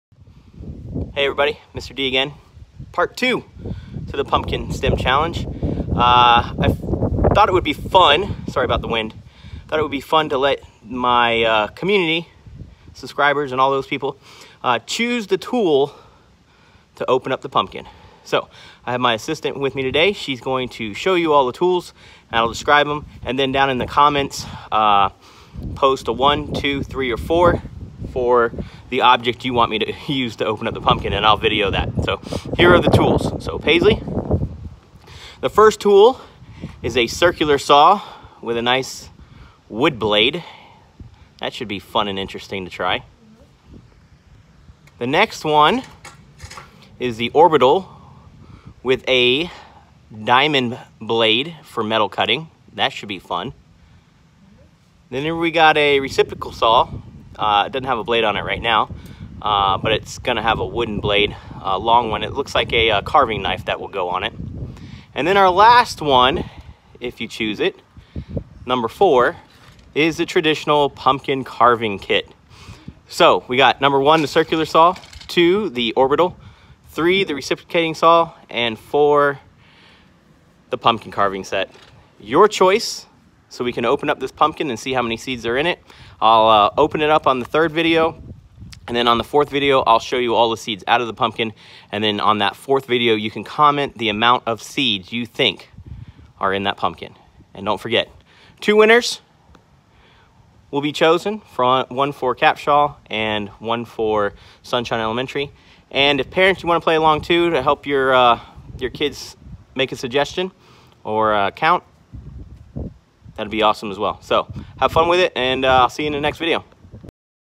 Pumpkin STEM challenge part 2
How many seeds are in this pumpkin? Choose the tool you would like to see me use in part 3.